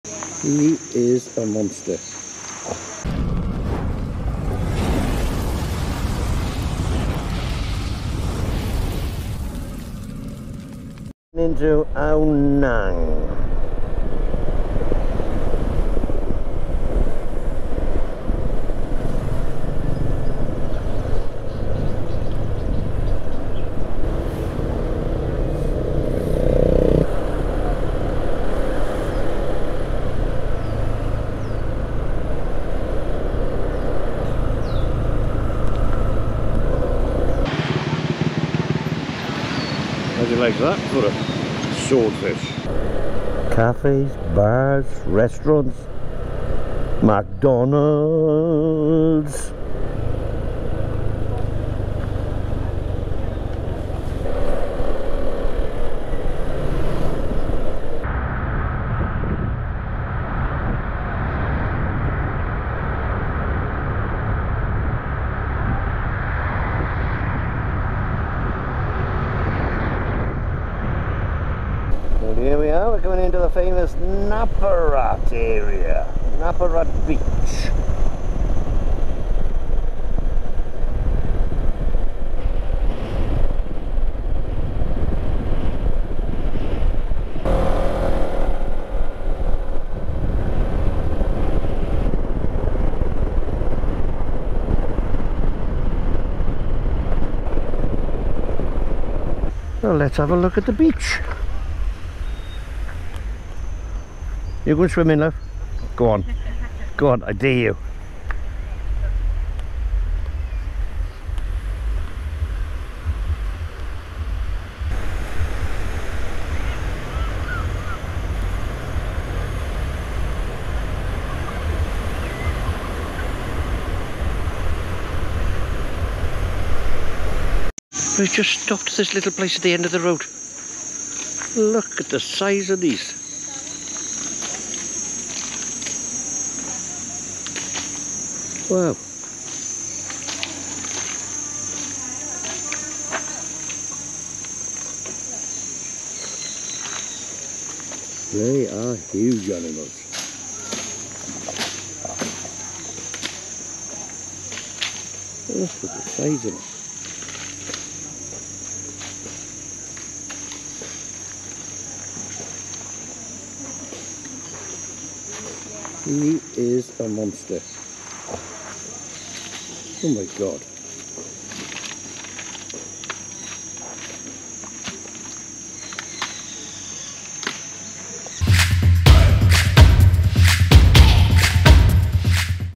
BEAUTIFUL ELEPHANTS
A trip around Napart and Ao Nang beach and we found some beautiful elephants that were not tethered or tied up :))